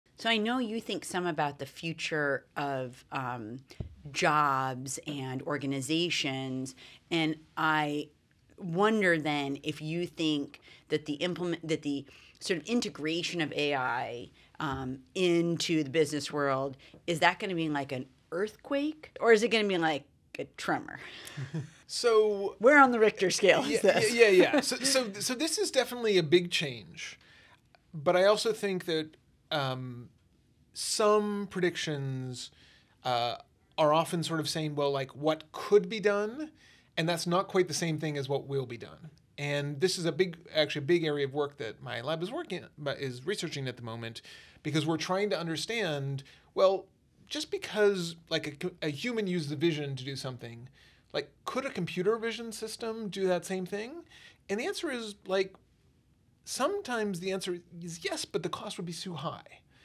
Will AI be an Earthquake or a Tremor for industry?
CSAIL’s Neil Thompson explains how AI will affect industry and the economy.